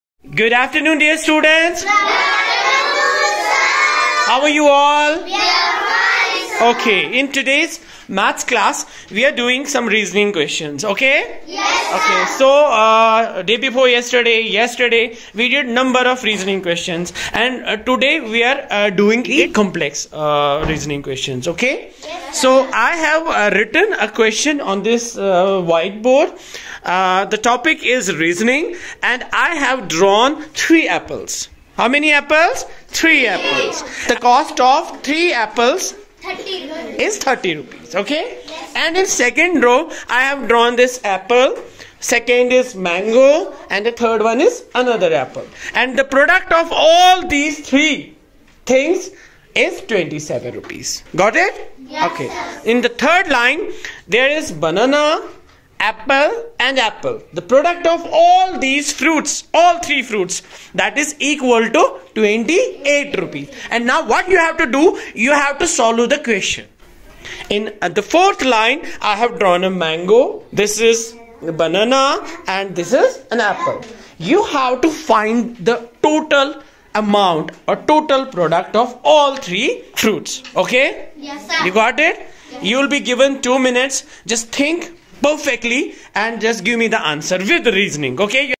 Hamara Government School | Hum Bachon Ko Tayar Karte Hain Na Ki Padhate Hain
School :- Government Girls Middle School Newcolony Pulwama Kashmir
Subject Maths
Topic :-Reasoning | Cognitive Questions
Objectives :-SWBAT
*Think and explain the statements with reasons
*Develop curiosity
*Develop the ability of thinking

We should provide opportunity and try to frame up such statements and questions so that the students may be able to think and Develop cognitive skills